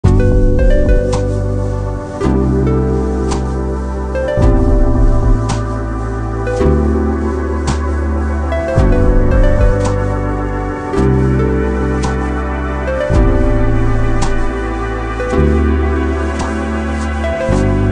A large underwater turtle flies in the blue ocean
Calm music for meditation and relaxation, A large underwater turtle flies in the blue ocean

You may like our channel with great relaxation videos